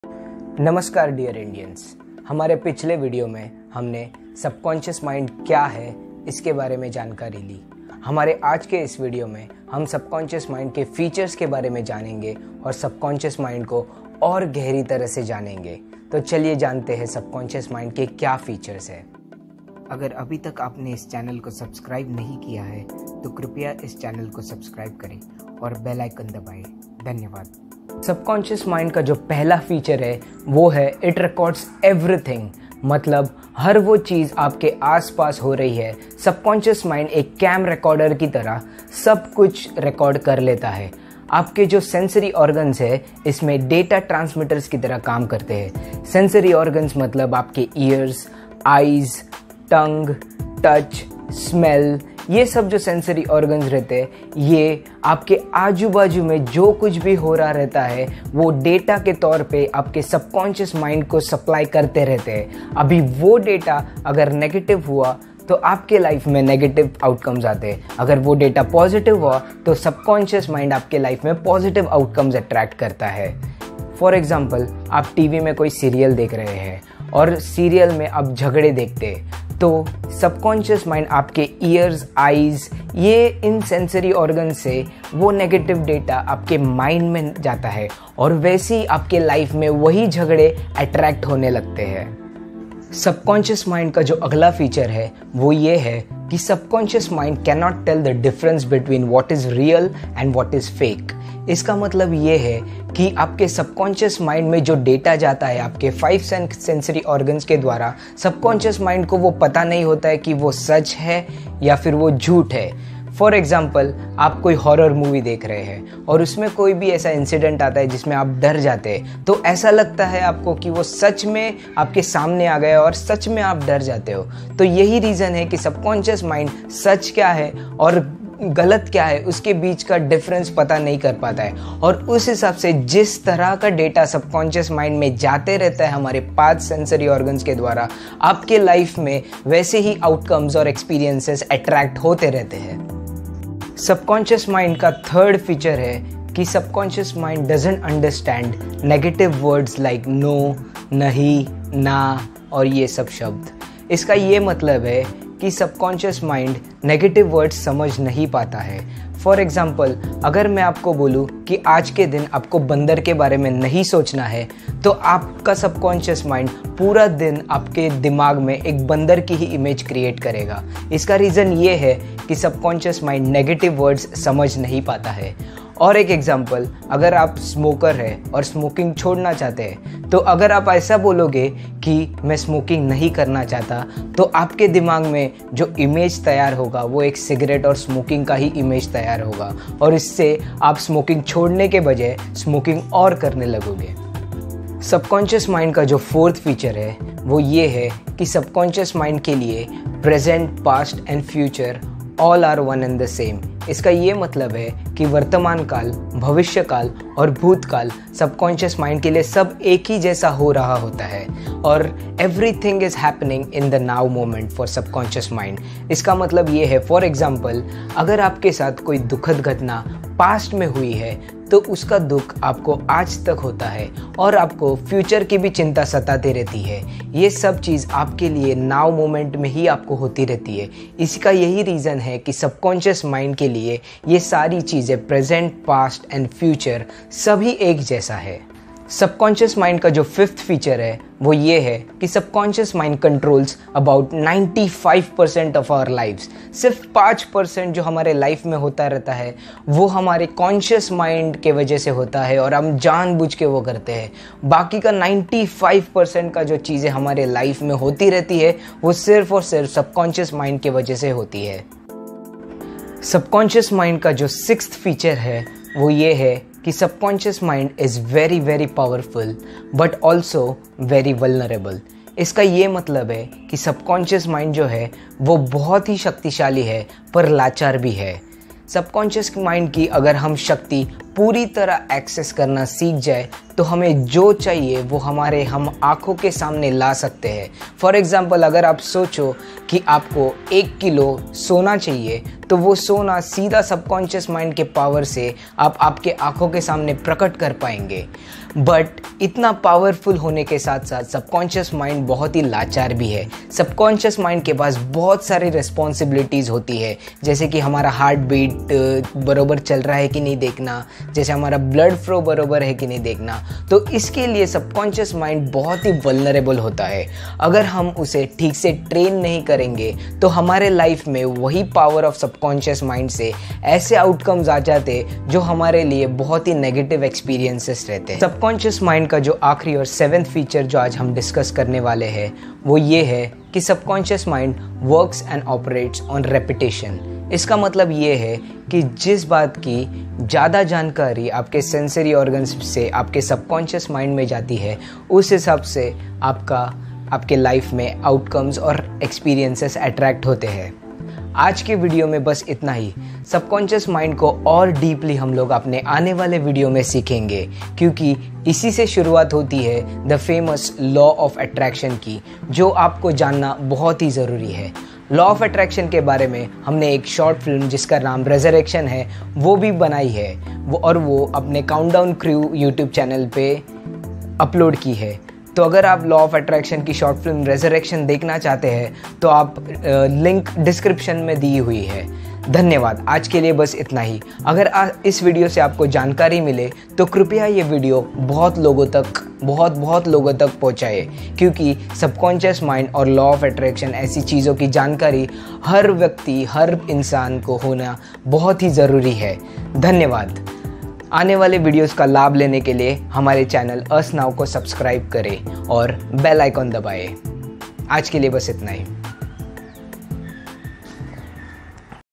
SUBCONSCIOUS MIND || FEATURES || YOU MUST KNOW || GGUS FOUNDATION || IMPORTANT VIDEO || MUST WATCH
In this Age of Information, Ignorance is a Choice.
In this video we have discuss the important features of our subconscious mind that you must know

Link to previous video - what is subconscious mind

Link to our Short film on Law of Attraction - Ressurection

IN THE NEXT VIDEO WE WILL DISCUSS ABOUT LAW OF ATTRACTION AND HOW TO REPROGRAM OUR SUBCONSCIOUS MIND